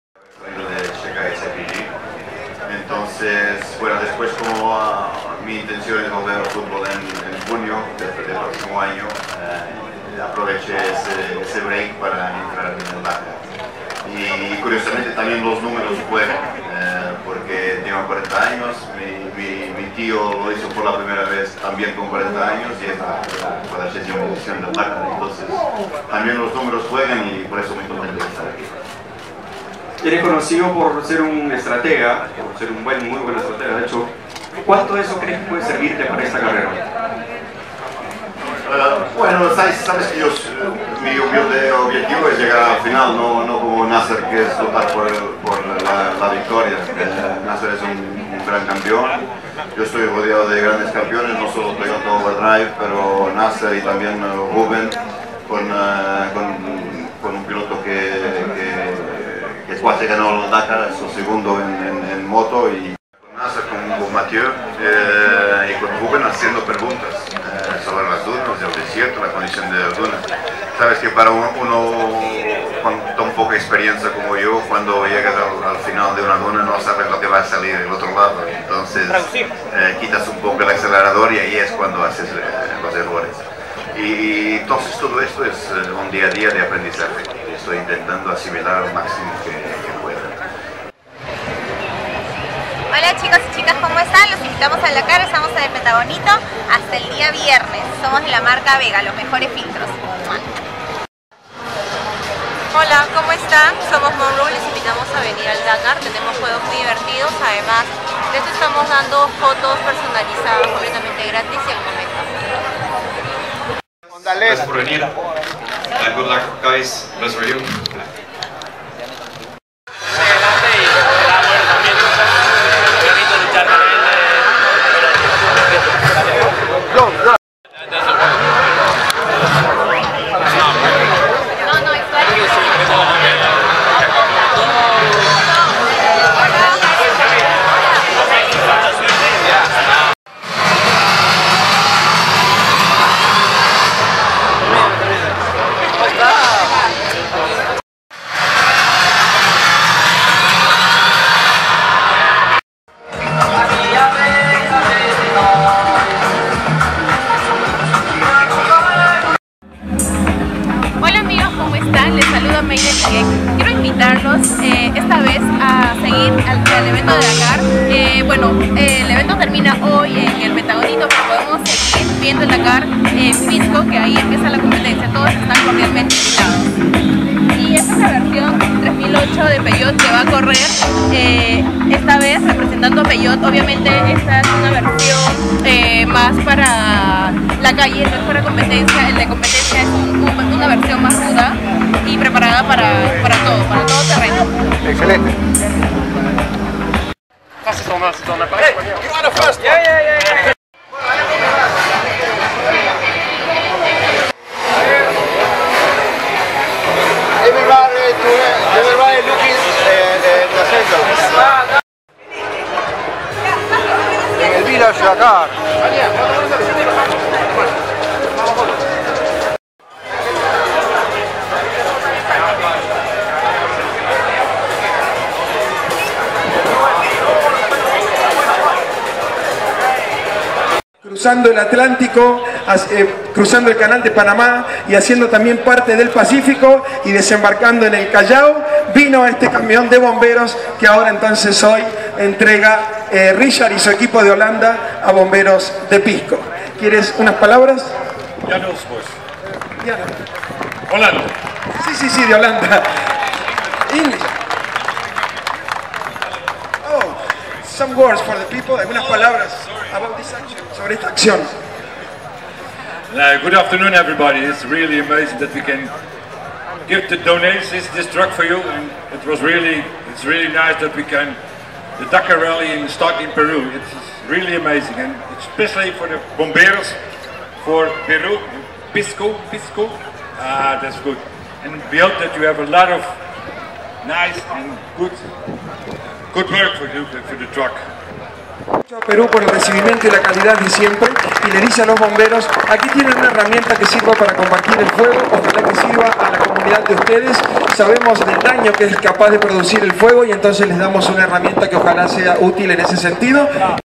Recorriendo el Village Dakar 2018 .Video reporte.- Javier Espichán Gambirazio
Recorriendo el Village Dakar 2018
En el Pentagonito de San Borja
Video reporte .-  Javier Espichán Gambirazio
Peru Tour International